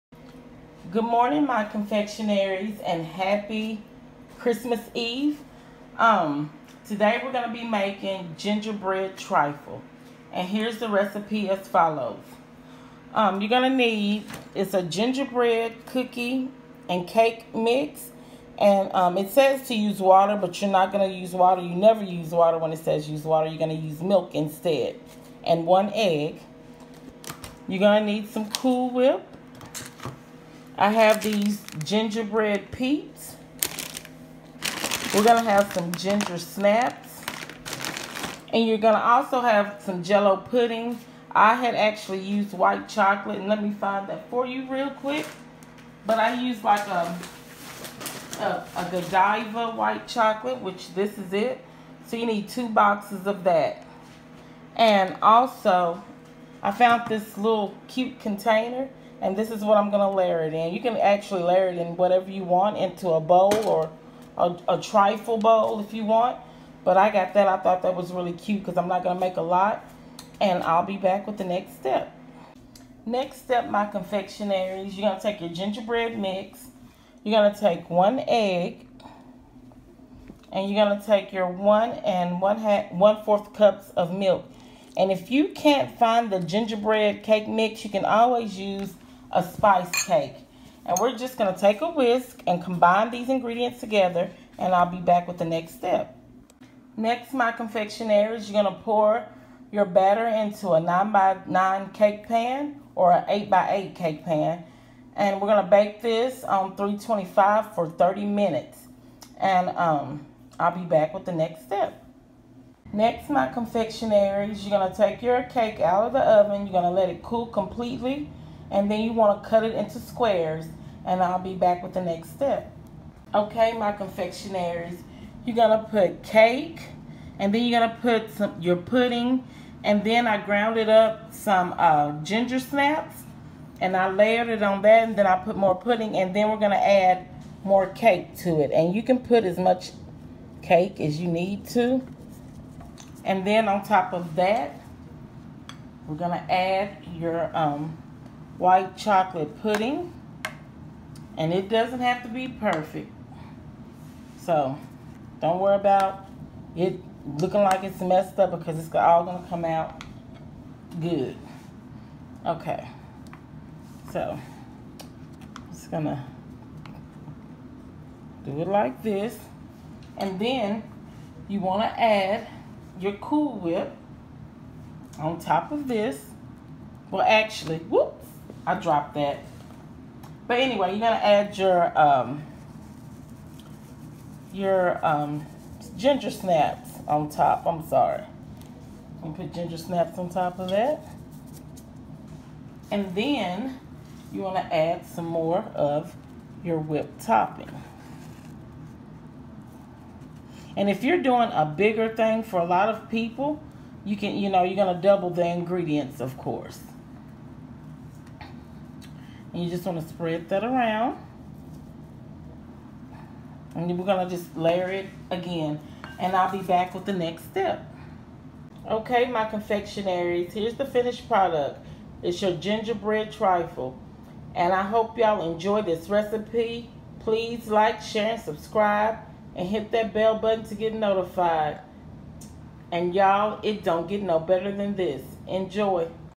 Gingerbread trifle with Godiva white chocolate pudding
1 box of gingerbread cake
White Godiva chocolate pudding
1 container of cool whip
Ginger snap cookies
Gingerbread man peep optional